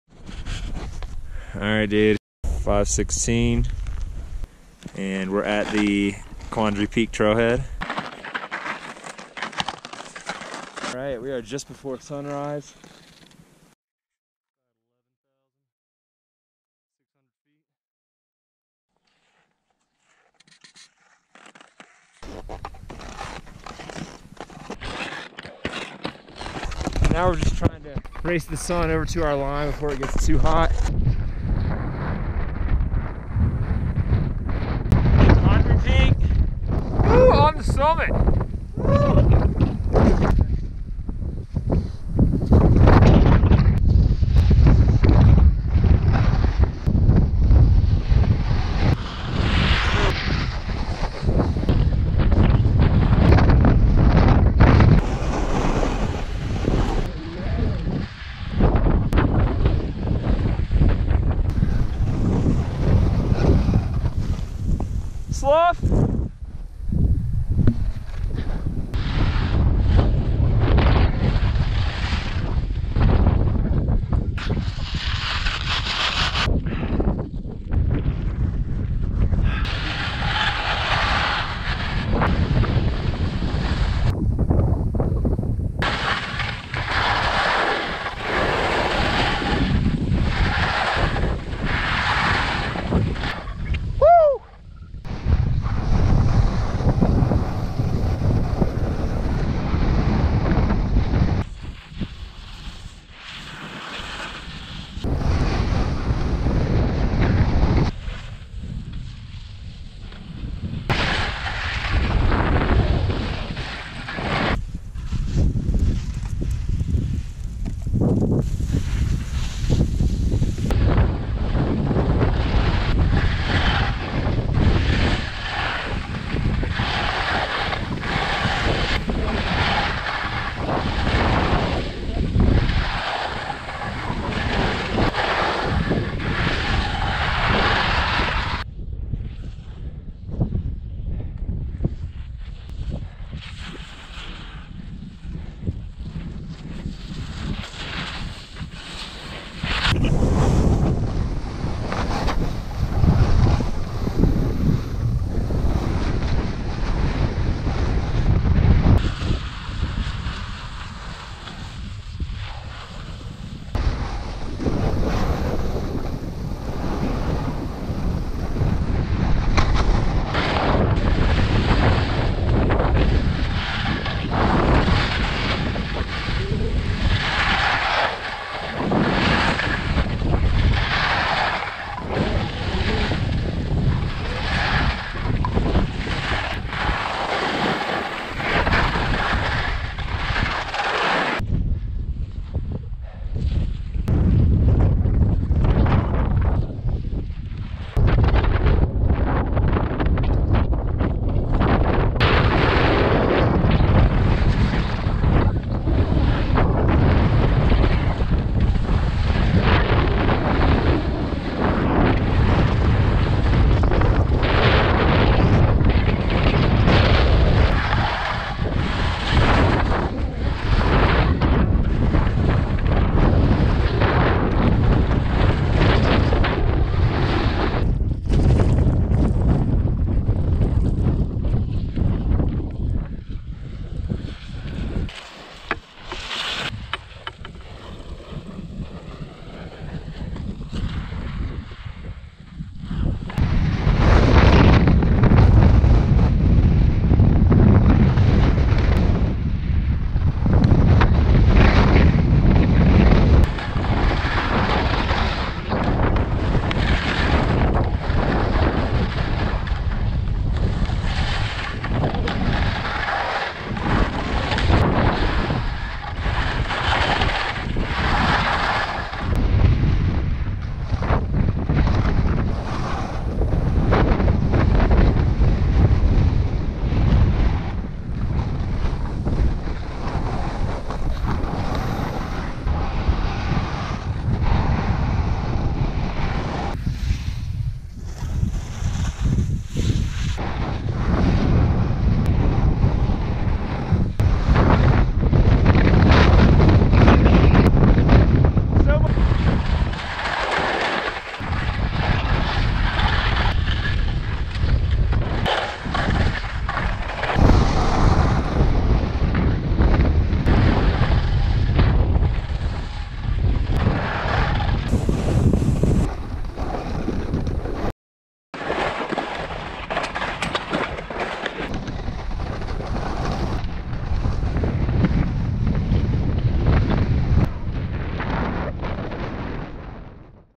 Cristo Couloir Ski - Quandary Peak - 14,256ft - Colorado
Ascent up the east ridge of Quandary Peak to 14,265ft, and skiing Cristo Couloir on the south face from the summit, descending over 2,500ft..